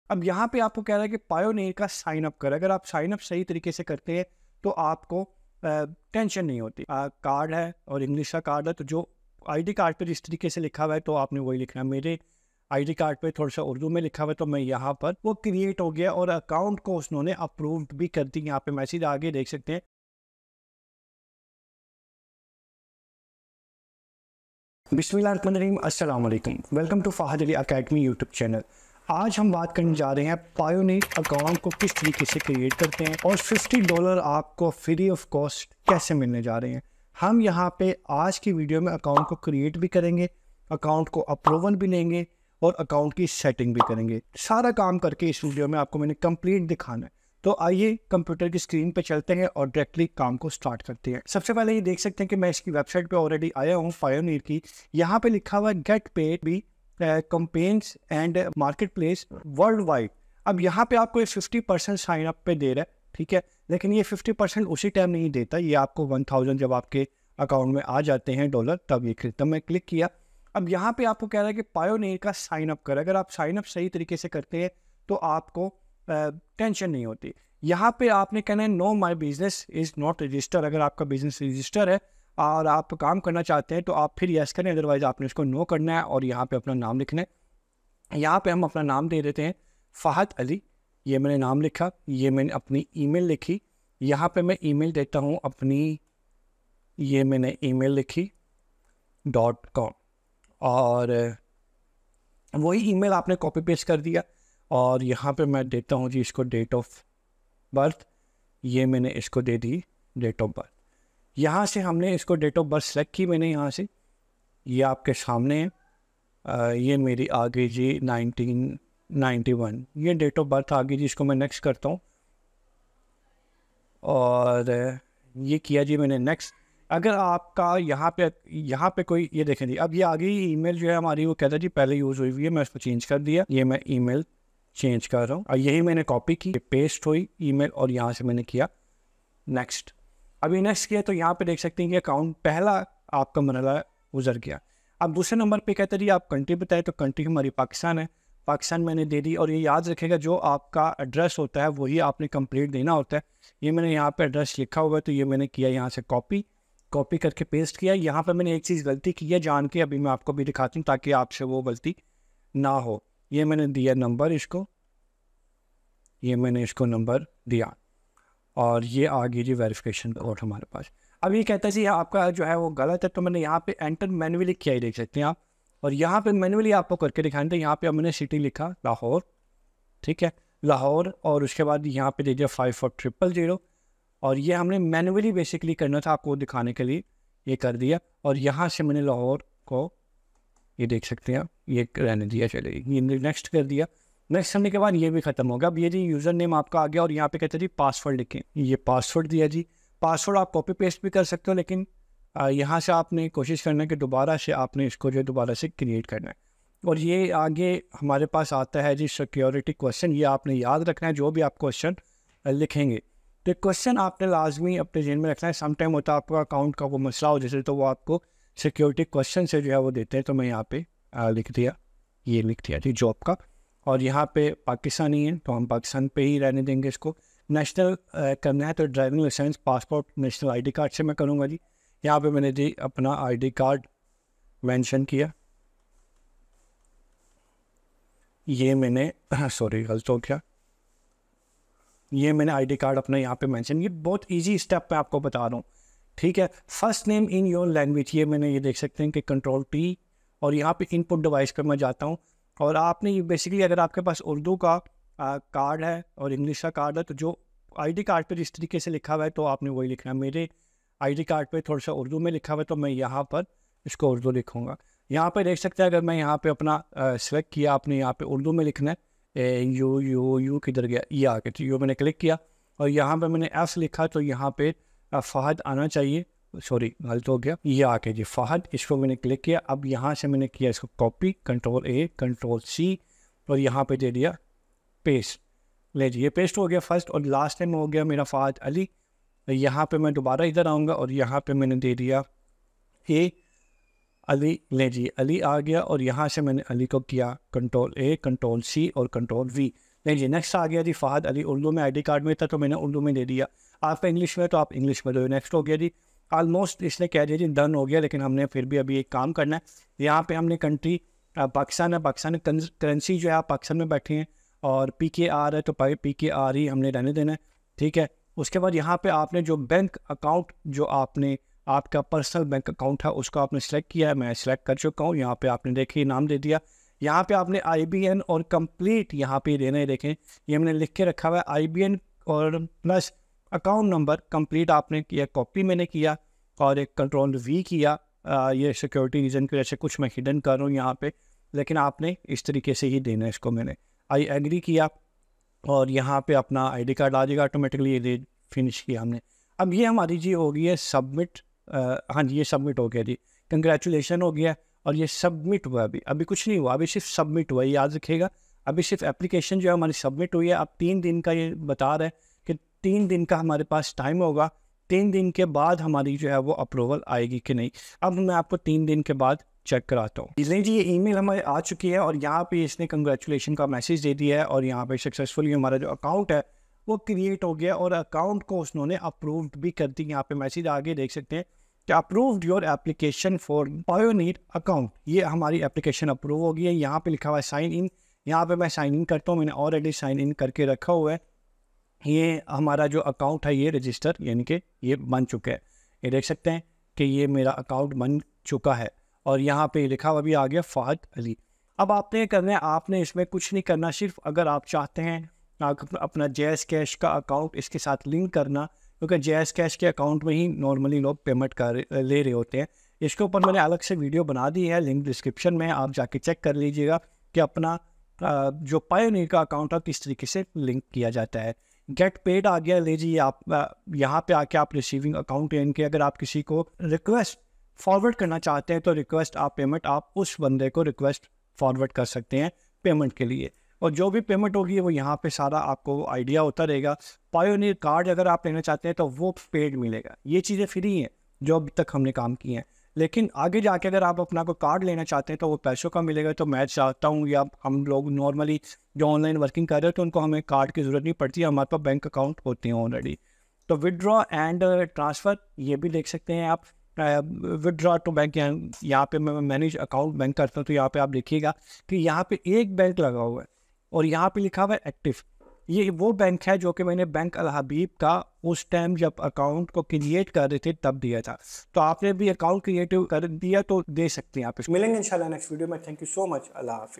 Easy way to create payoneer account in Pakistan | Pakistan Mein 2 din mein payoneer account approved
Creating a Payoneer account in Pakistan is a straightforward process. Here’s how you can do it:
1. **Visit the Payoneer Website**: Go to the official Payoneer website.
2. **Sign Up**: Click on the "Sign Up" button. You’ll have the option to choose between a personal or business account. Select the one that suits your needs.
4. **Provide Address and Phone Number**: Enter your residential address and a valid phone number. This information is important for verification.
5. **Verify Your Identity**: You may need to submit identification documents such as a passport or national ID, as well as proof of address. Ensure these documents are clear and legible.
6. **Bank Account Information**: You’ll need to link your local bank account for withdrawals. Make sure to provide accurate banking details.
8. **Submit Your Application**: Once everything is filled out, submit your application. You should receive a confirmation email shortly.
9. **Wait for Approval**: Payoneer will review your application. This process may take a few days, and you’ll receive an email regarding your account status.
10. **Start Using Your Account**: Once approved, you can log in to your Payoneer account and start using it for transactions.

Related Links